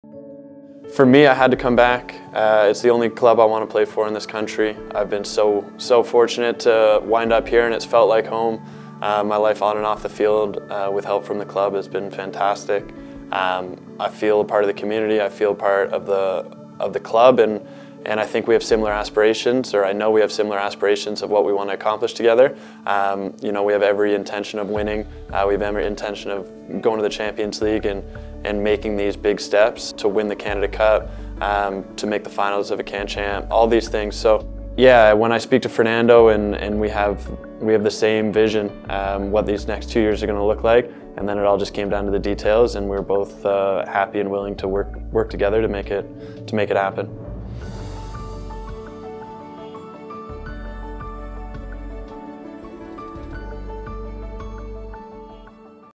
Atlético Ottawa Goalkeeper Nathan Ingham Talks About Re-Signing ✍️
Atlético Ottawa today announced the club has re-signed Nathan Ingham to a new contract that will keep the Canadian goalkeeper in the nation’s capital through the 2025 Canadian Premier League (CPL) season.

“Re-signing Nate was a priority for us this off-season, so I am thrilled to be announcing his return,” said Fernando Lopez, CEO, Atlético Ottawa.

“When Nate first arrived in Ottawa, I said he fit our club’s values and style very well because of his pure shot stopping ability, how aggressively he plays for every ball and the great character he shows on and off the field. Nate has proven all of that and more since joining our club, which is why it was imperative we brought him back for another two years.”

Ingham, 30, signed with Atlético Ottawa in January 2022, ahead of the club’s third season in the CPL. The native of Keswick, ON had already established himself as one of the best goalkeepers in the CPL prior to his arrival, playing 62 matches and engineering 17 clean sheets for York United FC over the league’s first three seasons. He was rewarded with a CPL Golden Glove nomination, awarded to CPL goalkeeper voted best at his position, in 2019.